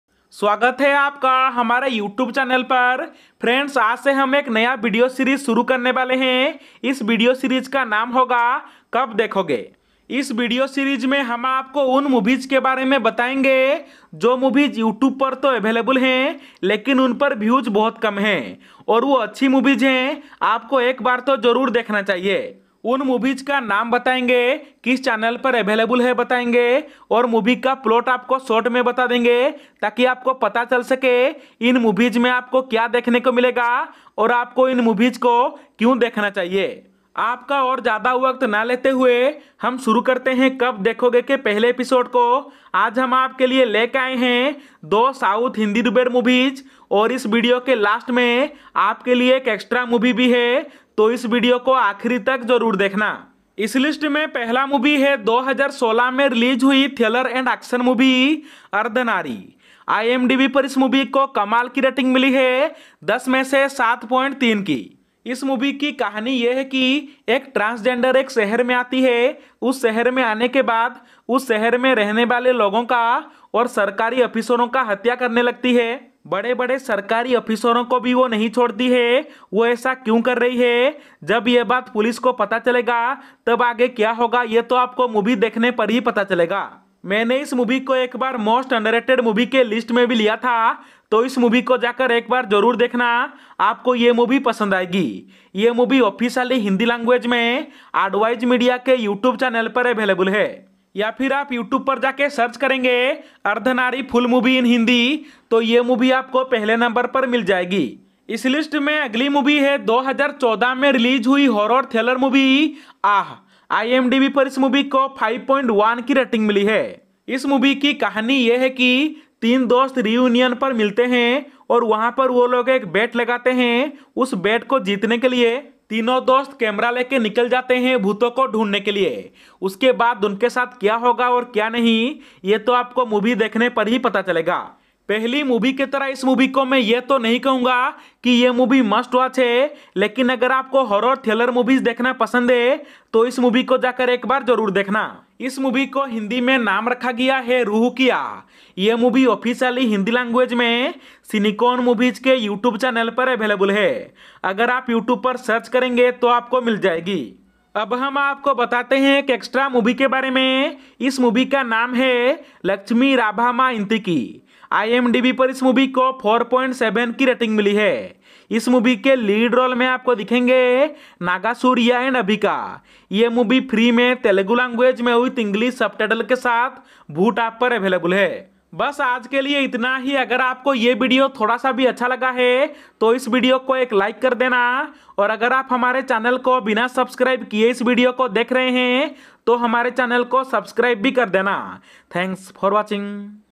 2 Best South Hindi Dubbed Movies Available On YouTube | KAB DEKHOGE #1
KAB DEKHOGE #1 - In This Video I Will Talk About 2 Best South Hindi Dubbed Movies Available On YouTube

"KAB DEKHOGE"  Is A Video Series.
In This Video Series, We Will Tell You About Those Movies Which Are Available on YouTube But Not Many People Know About Those Movies.
Our objective with this video series is that we tell people about those movies.

If We Support Good South Films Right Now, So In The Future, Similar Good South Films Will Continue To Come.